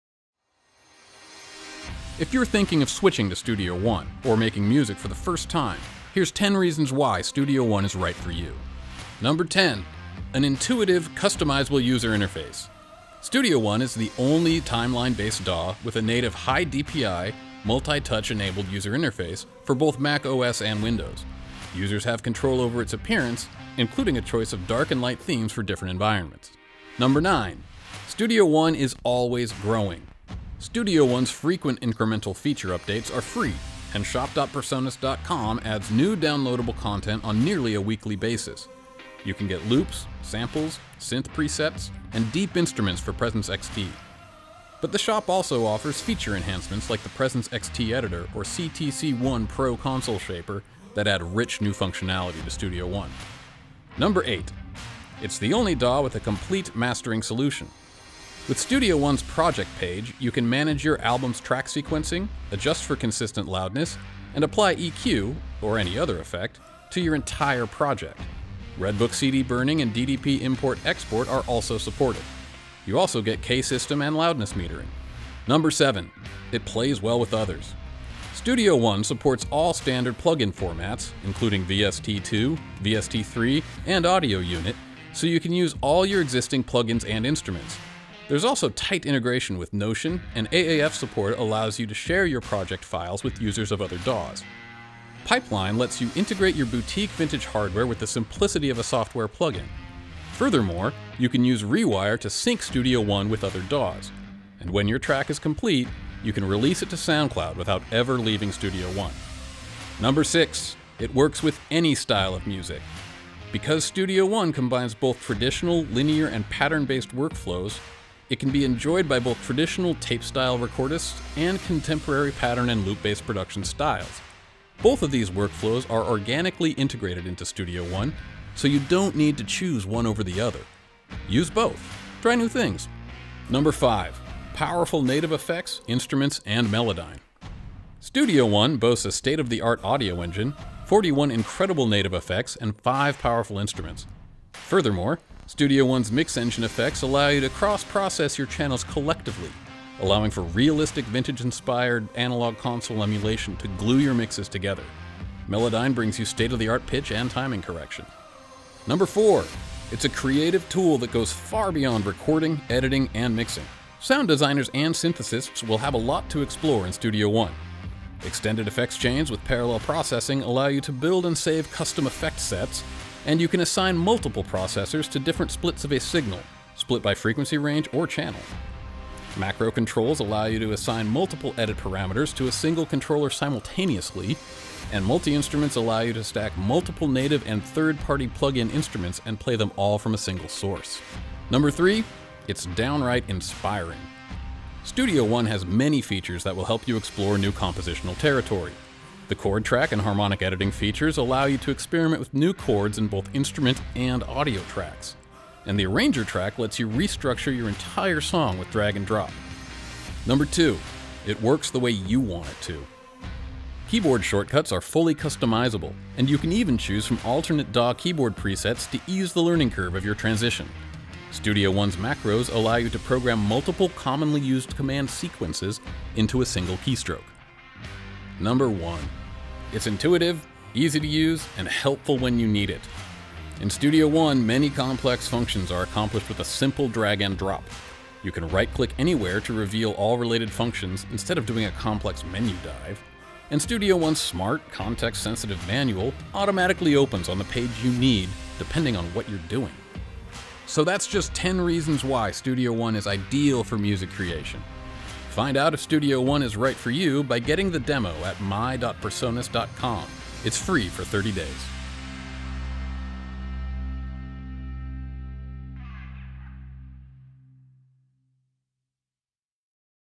PreSonus—10 Reasons Why Studio One Is Right for You
It doesn't matter if you're interested in switching DAWs or making music for the first time ever—here's ten reasons why Studio One is the right DAW for you.